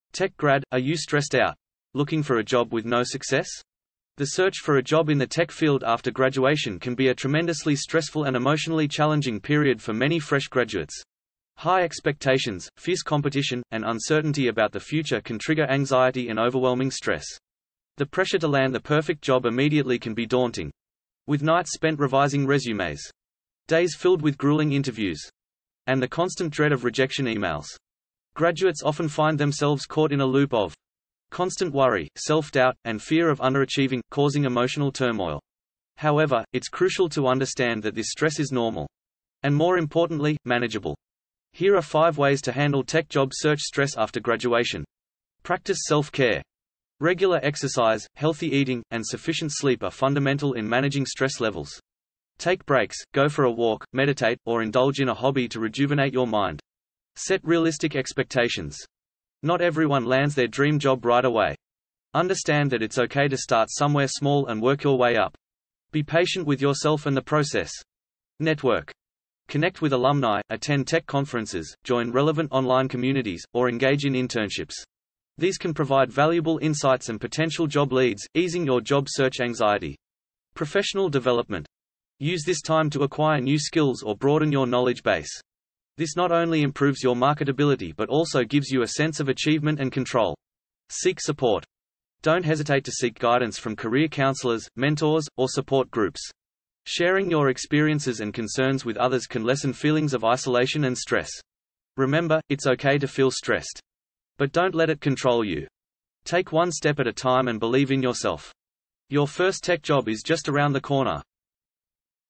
Secrets To Relieve Job Search Stress: Tech Grads
The search for a job in the tech field after graduation can be a tremendously stressful and emotionally challenging period for many fresh graduates. High expectations, fierce competition, and uncertainty about the future can trigger anxiety and overwhelming stress.

The pressure to land the perfect job immediately can be daunting, with nights spent revising resumes, days filled with grueling interviews, and the constant dread of rejection emails. Graduates often find themselves caught in a loop of constant worry, self-doubt, and fear of underachieving, causing emotional turmoil.

However, it's crucial to understand that this stress is normal, and more importantly, manageable. Here are five ways to handle tech job search stress after graduation:

Practice Self-Care: Regular exercise, healthy eating, and sufficient sleep are fundamental in managing stress levels. Take breaks, go for a walk, meditate, or indulge in a hobby to rejuvenate your mind.

Set Realistic Expectations: Not everyone lands their dream job right away. Understand that it's okay to start somewhere small and work your way up. Be patient with yourself and the process.

Network: Connect with alumni, attend tech conferences, join relevant online communities, or engage in internships. These can provide valuable insights and potential job leads, easing your job search anxiety.

Professional Development: Use this time to acquire new skills or broaden your knowledge base. This not only improves your marketability but also gives you a sense of achievement and control.

Seek Support: Don’t hesitate to seek guidance from career counselors, mentors, or support groups. Sharing your experiences and concerns with others can lessen feelings of isolation and stress.

Remember, it's okay to feel stressed. But don't let it control you. Take one step at a time and believe in yourself. Your first tech job is just around the corner.